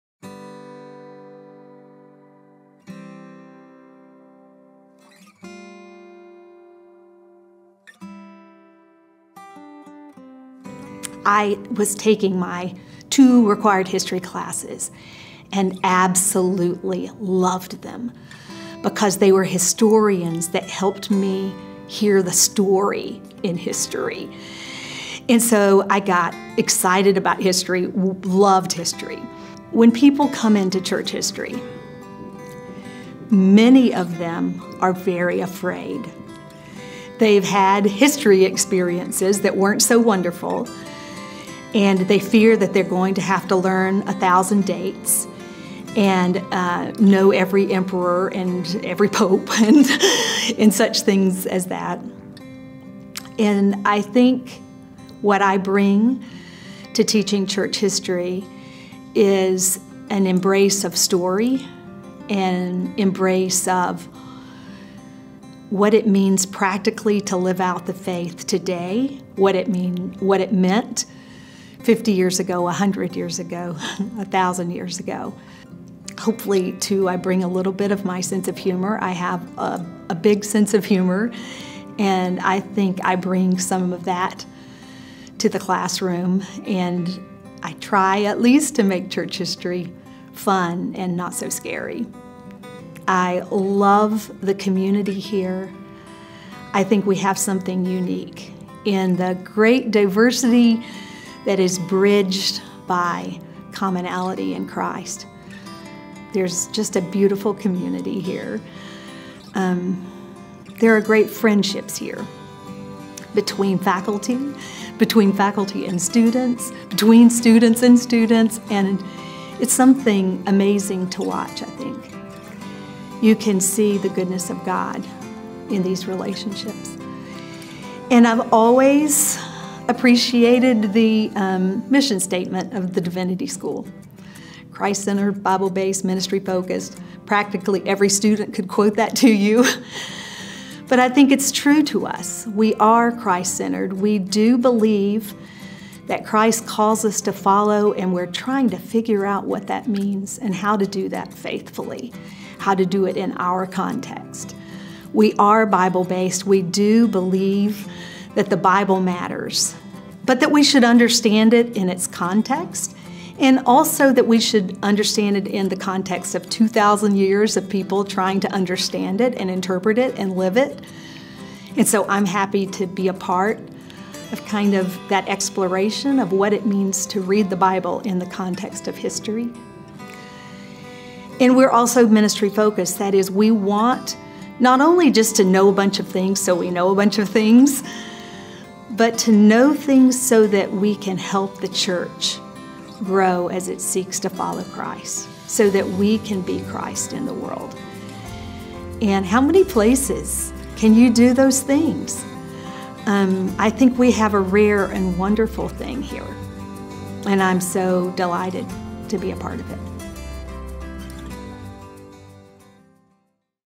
Faculty Spotlight: Dr. Lydia Hoyle
Dr. Lydia Hoyle, Associate Professor of Church History & Baptist Heritage

Before her retirement from Campbell University Divinity School in 2023, Dr. Lydia Hoyle served as an associate professor of church history and Baptist heritage. Prior to arriving at Campbell in 2003, she was a religion professor at Georgetown College, Georgetown, Ky. Her research interests and publications have included work in the areas of women and missions, children and mission education, Baptist history and women in ministry.